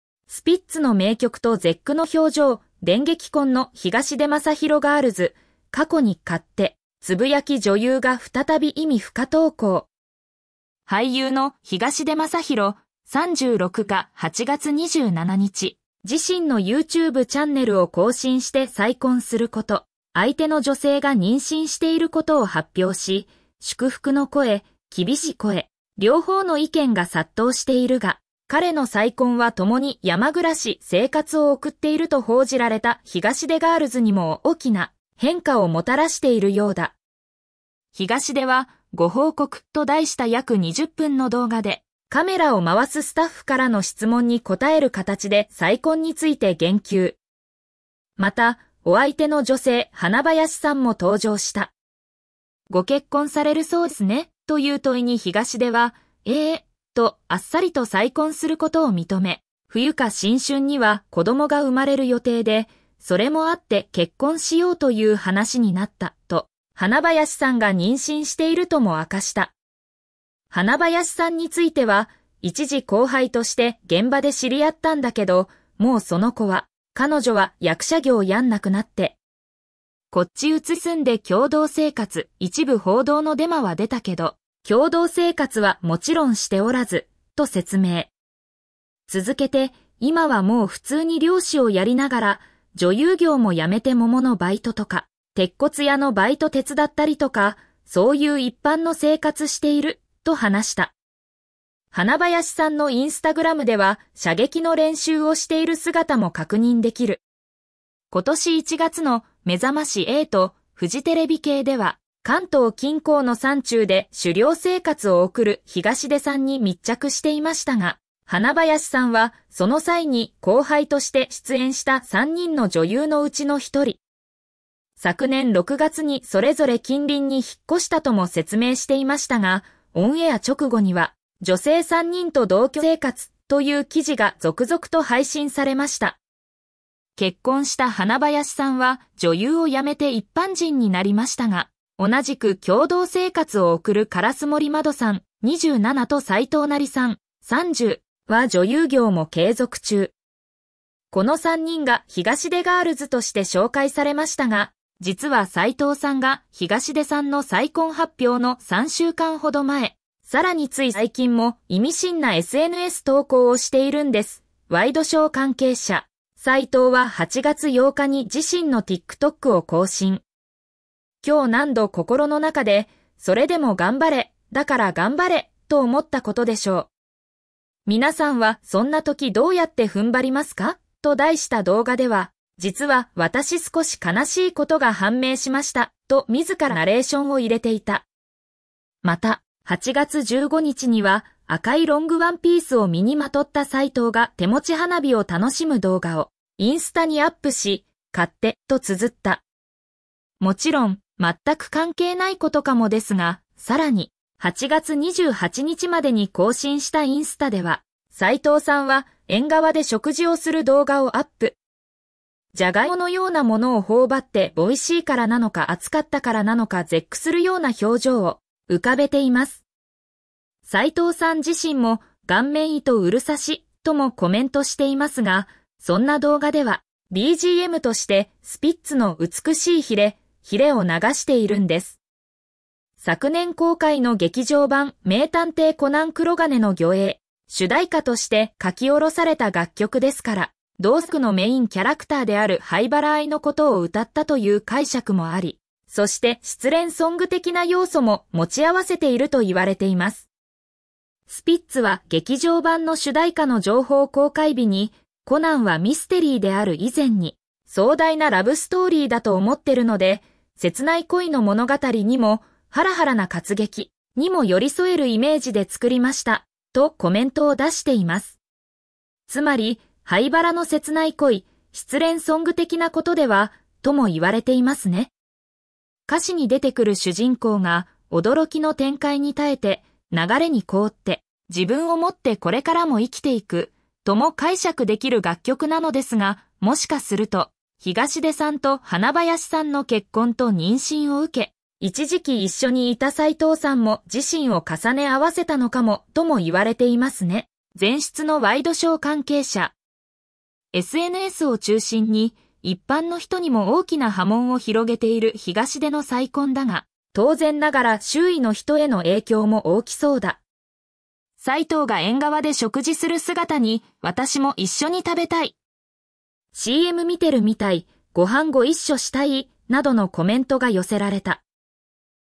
"スピッツの名曲と絶句の表情…東出昌大の電撃婚“ガールズ”、過去のツイート再浮上"
"東出昌大の突然の結婚が話題に！彼の結婚相手が過去に「勝手」とつぶやいた投稿が再び注目されています。さらに、スピッツの名曲が流れる中での絶句の表情とは？この動画では、話題の真相に迫りつつ、彼女の過去のSNS投稿について詳しく解説します。スピッツファンや芸能ニュースに興味がある方は必見です。"